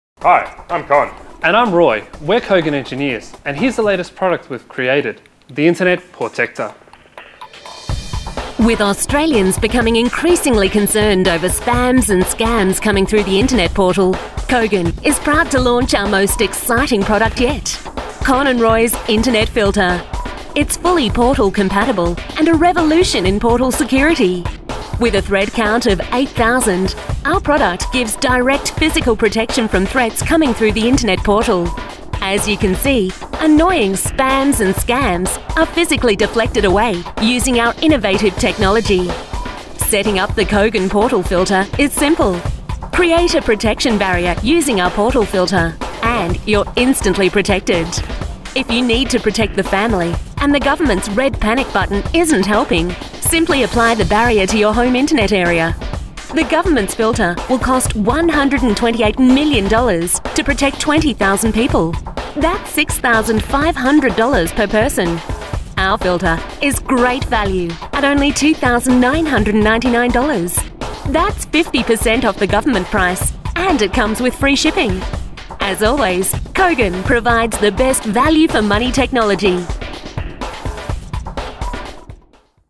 Kogan Portector - Spams & Scams Internet Portal Filter
Two budding engineers from the Kogan Technologies R&D team in Melbourne, Con & Roy, have developed an Internet filter that portects Australians from "spams and scams coming through the portal."

Stay up to date with all the latest Kogan news and future revisions of the Portector on Twitter